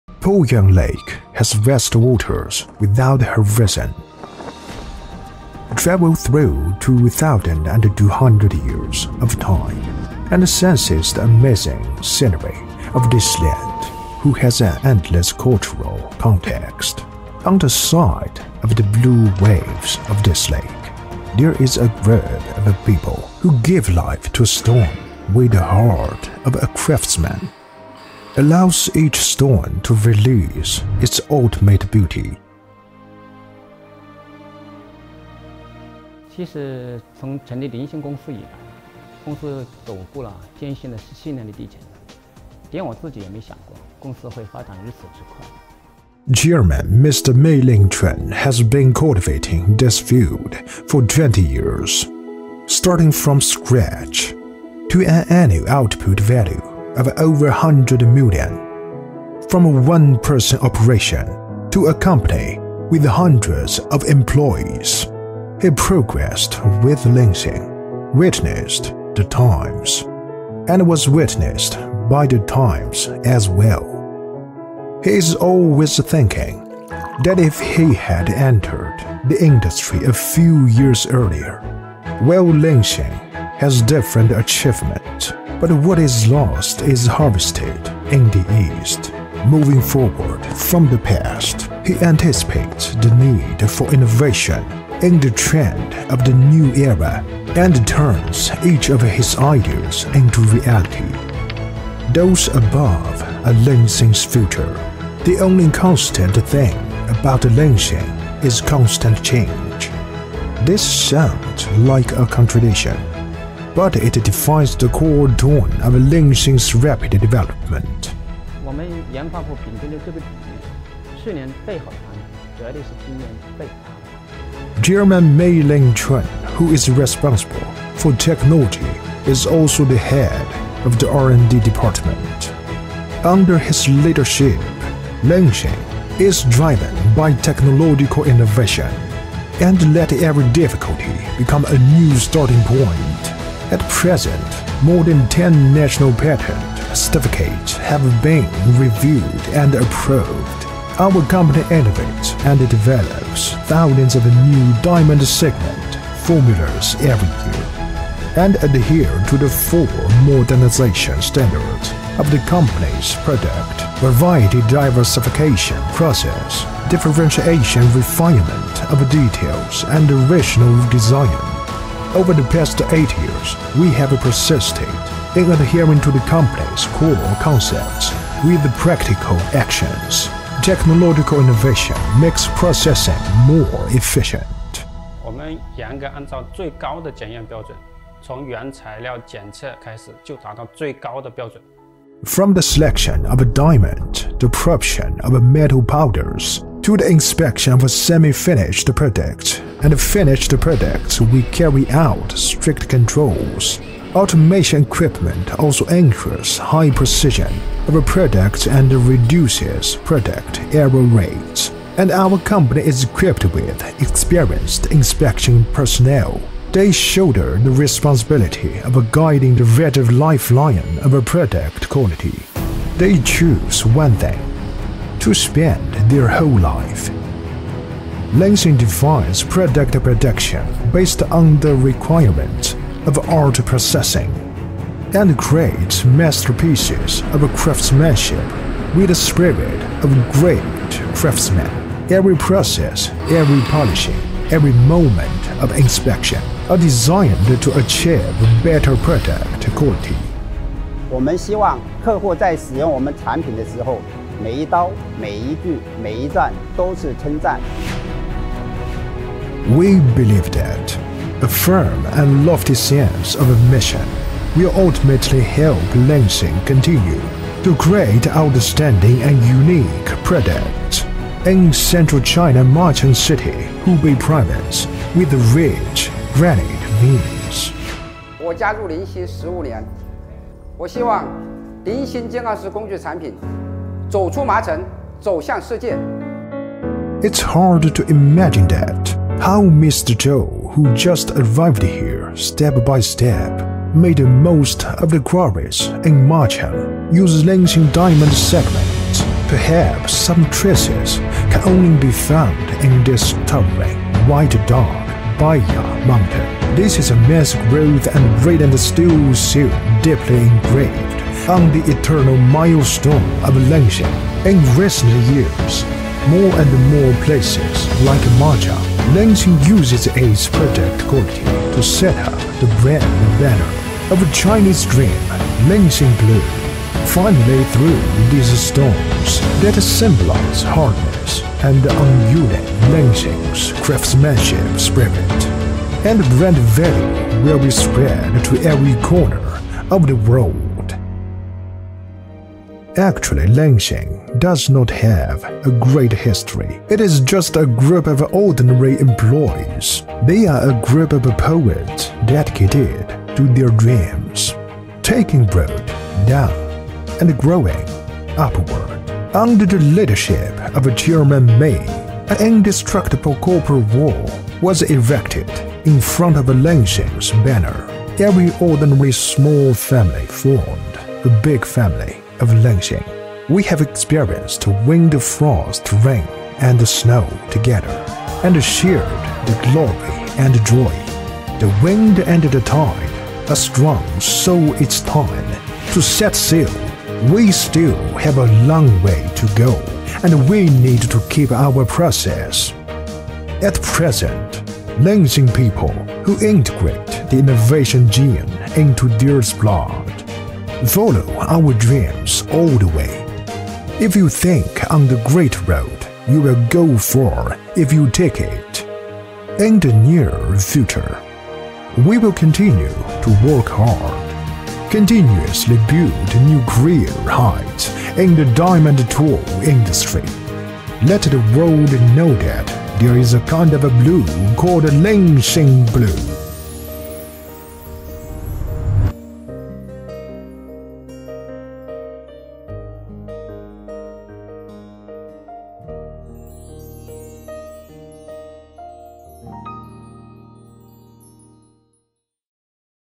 Jiangxi Linxing Diamond Tools Co., Ltd. corporate video
Jiangxi Linxing Diamond Tools Co., Ltd. was established in 2003 (officially renamed in 2014, formerly known as "Linxing Tool Factory"), and is located in the Industrial Park of Tianfan Street Town, Poyang County, Shangrao City, Jiangxi Province. It is a professional enterprise integrating R&D, design, manufacture and sales of diamond tools, stone machinery and other stone processing accessories. The main products include diamond segment, diamond saw blade, diamond wire saw, stone abrasive, stone profile processing tools and stone machinery, etc. which help stone enterprises solve many problems such as cutting, grinding, milling, profiling and drilling.